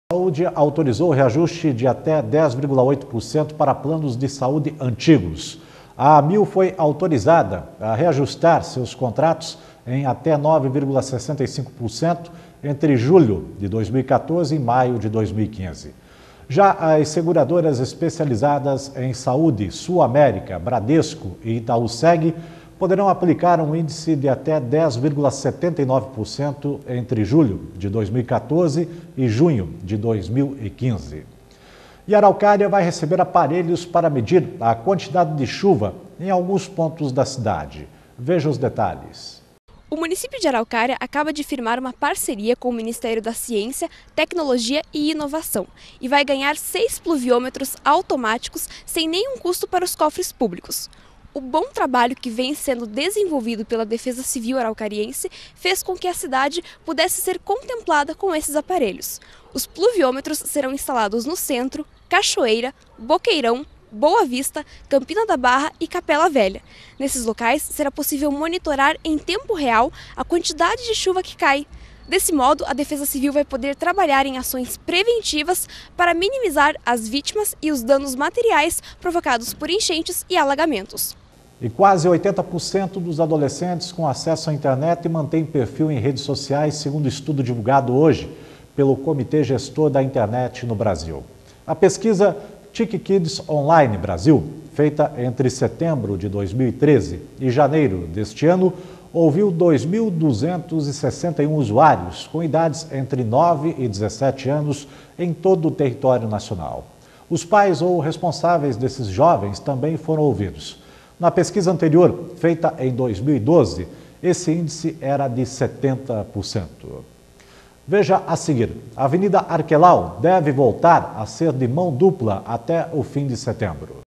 JR 06 08 14 Parte 2
Araucária vai ganhar aparelhos para medir a quantidade de chuva em seis bairros.